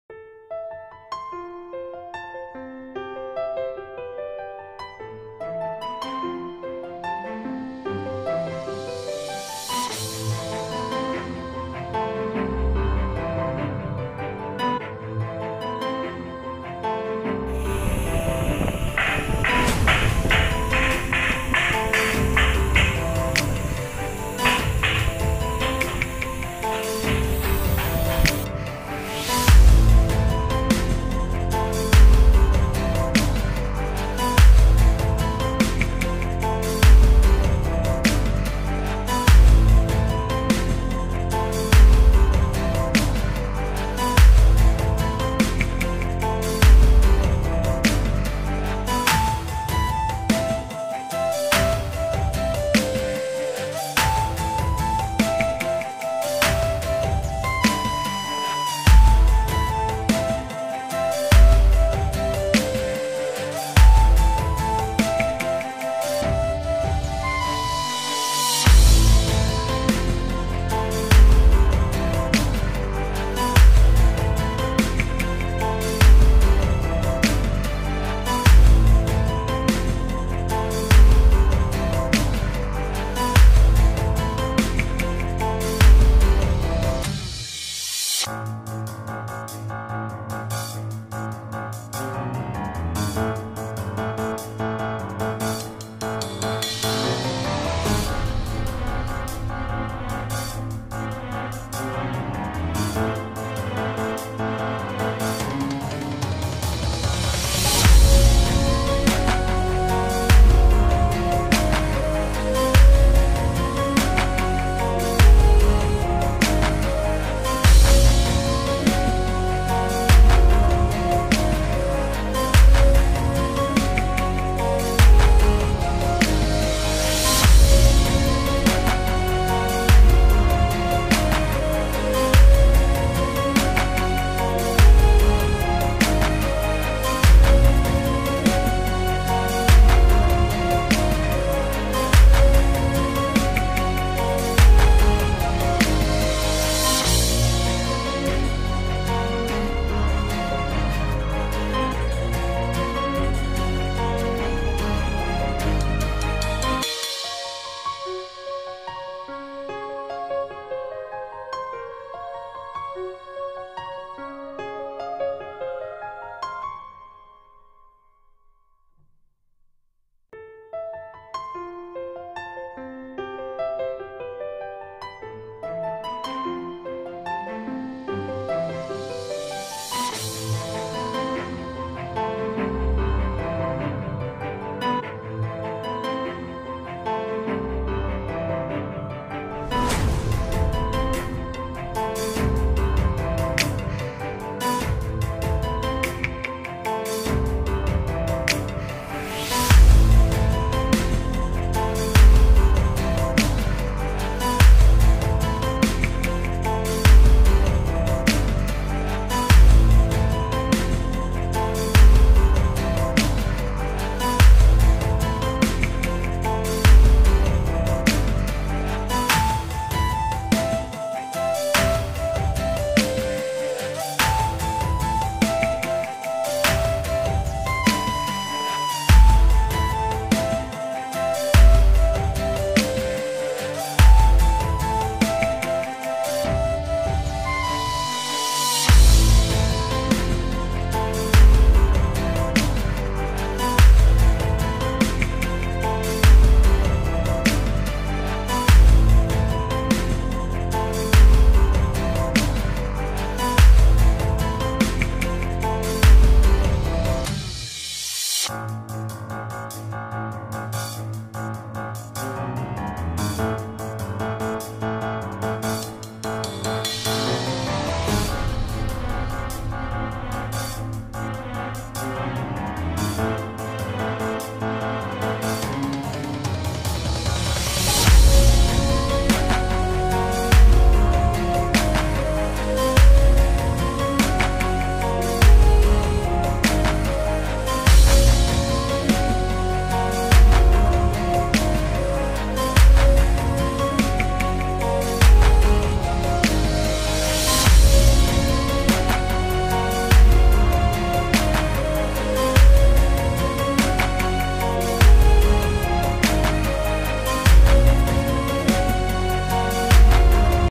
LONG DRILL NO VIBRATION/BORING BAR TOOL
an easy way to drill long on a lathe, so that there is no vibration in the holder then use a long drill tool .
Boring bar long tool.